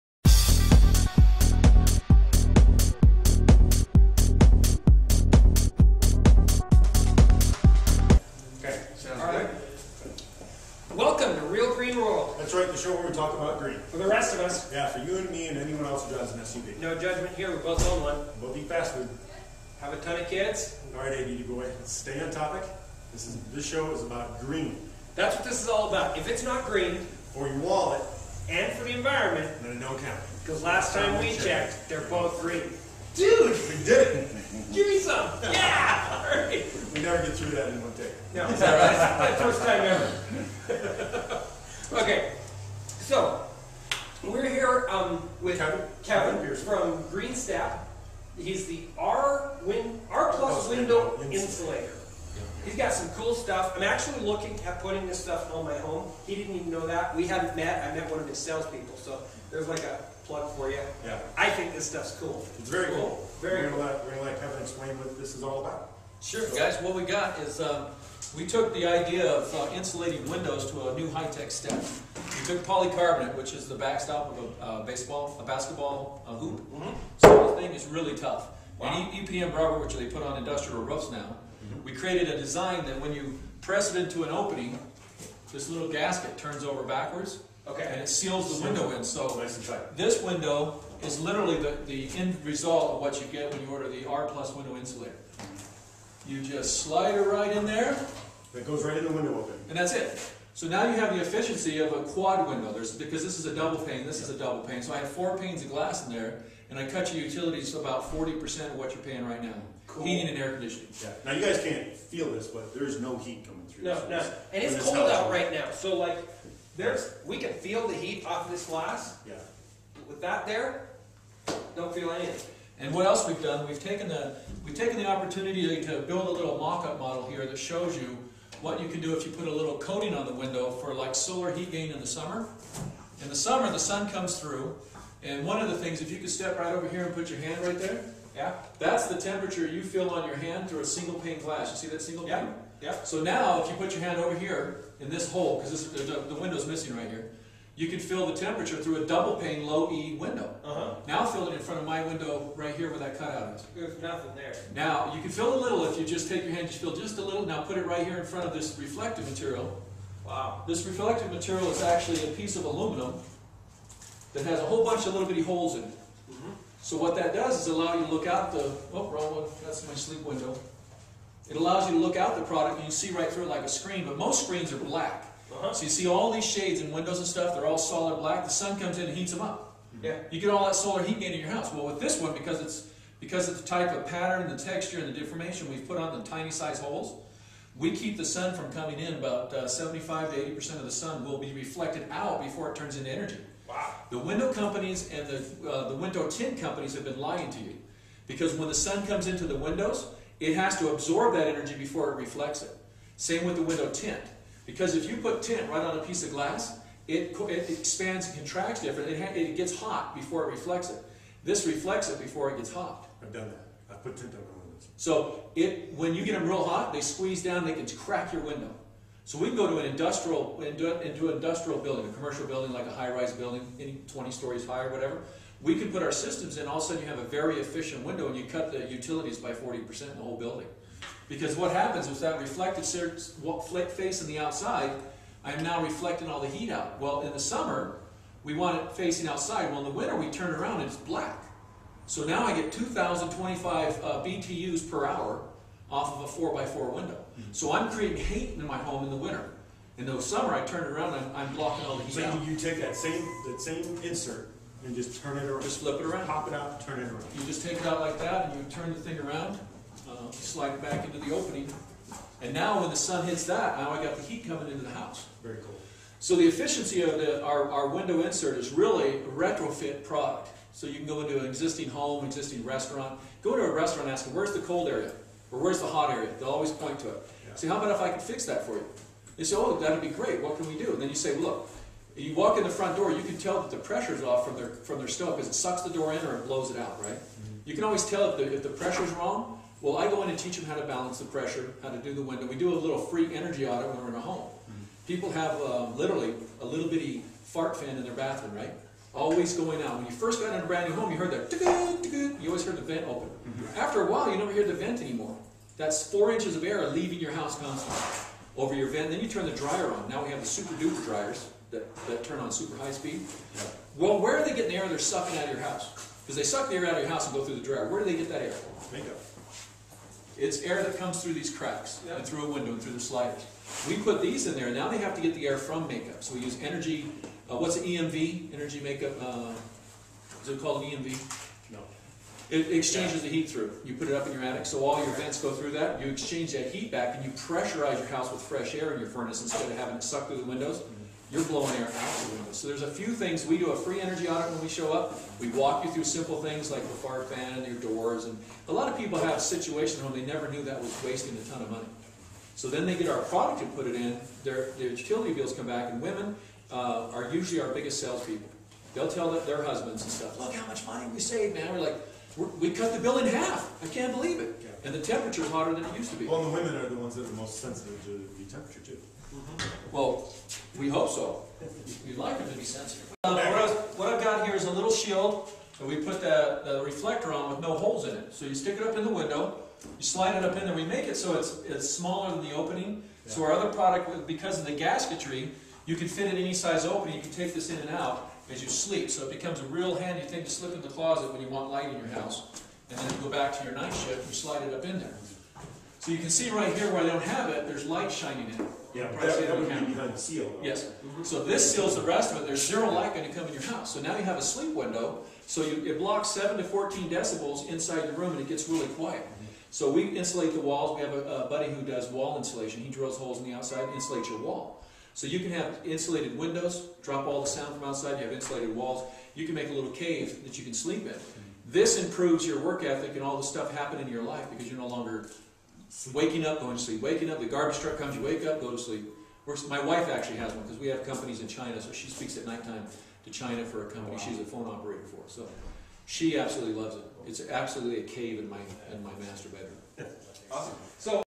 RGW 110512 R+ Windows.flv
Talk window inserts with the guys from R+ Windows. This is an interesting product. Thermal insulation through windows and doors is one of the best ways to improve energy efficiency. These are easy to install and cost effective to purchase.